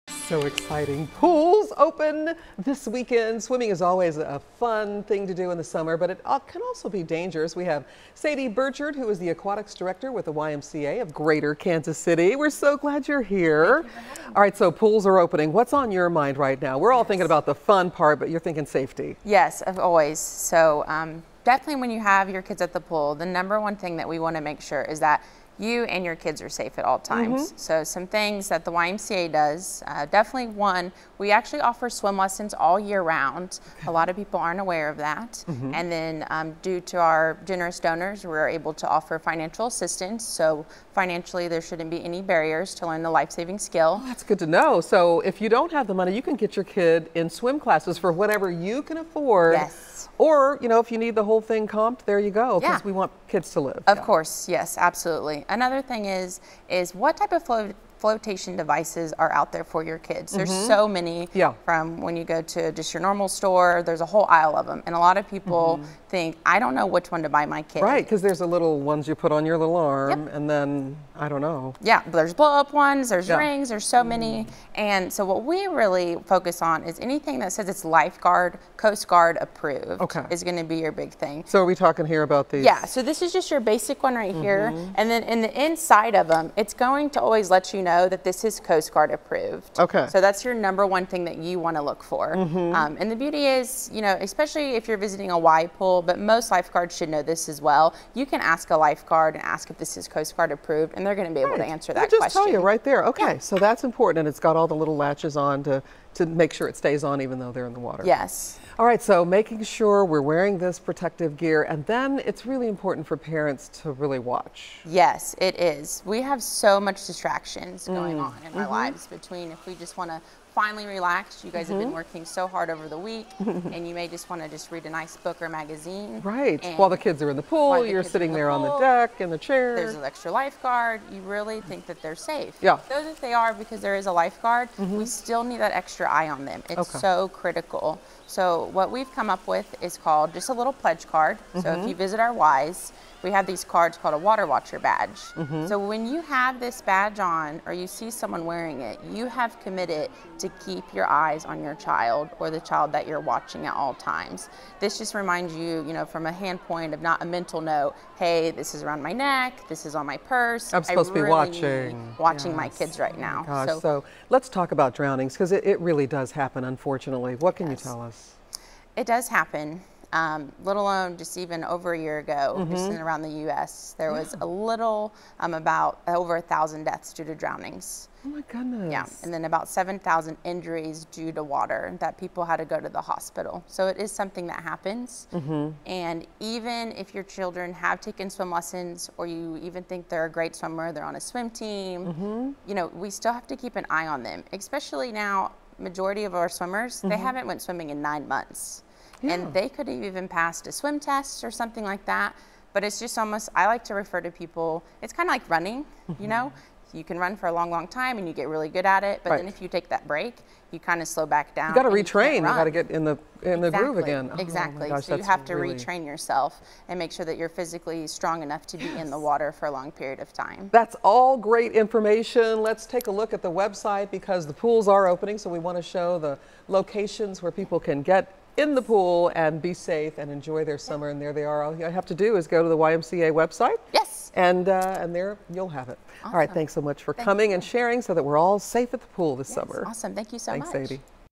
Keeping kids safe while they swim
Pools open this weekend. Swimming is fun in the summer, but it can also be dangerous. Sadie Birchard, the Aquatics Director with the YMCA of Greater Kansas City, talks swimming safely.